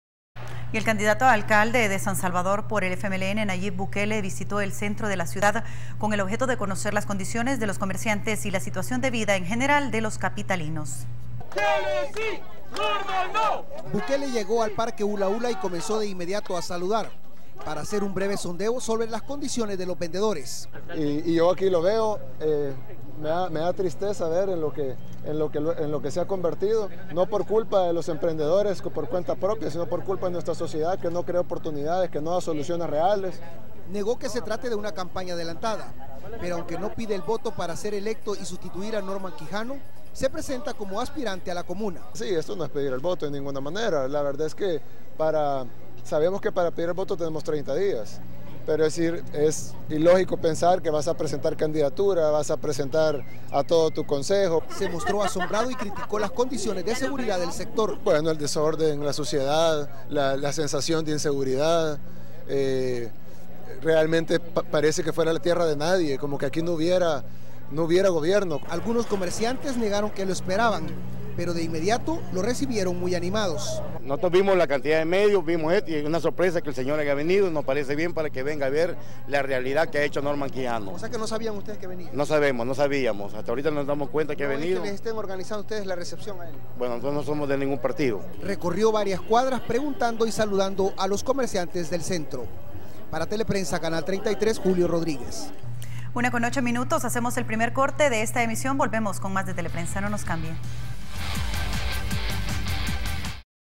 Nayib Bukele visita el centro de la capital
El candidato alcalde de San Salvador por el FMLN, Nayib Bukele, visito el centro de la ciudad con el objeto de conocer las condiciones de los comerciantes y la situación de vida en general de los capitalinos.